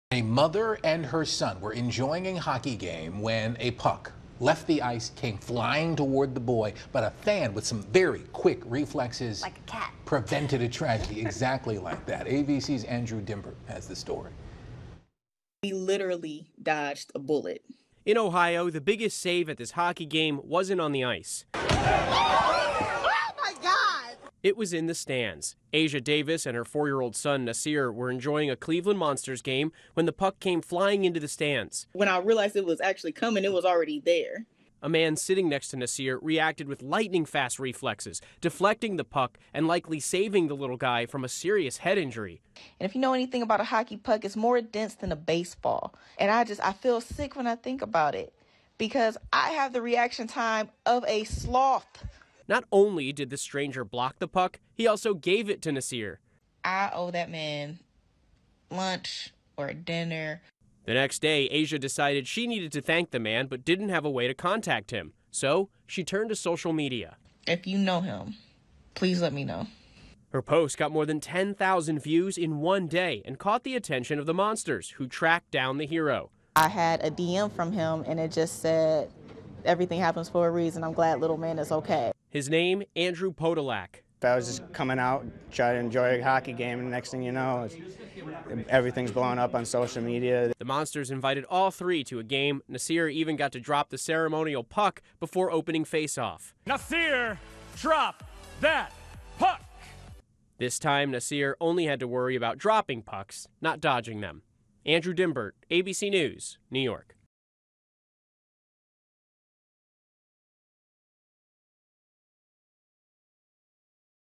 Hero hockey fan blocks puck from hitting 4-year-old boy
A fun night at the hockey rink could have had a tragic ending, if not for a speedy Good Samaritan who saved the day.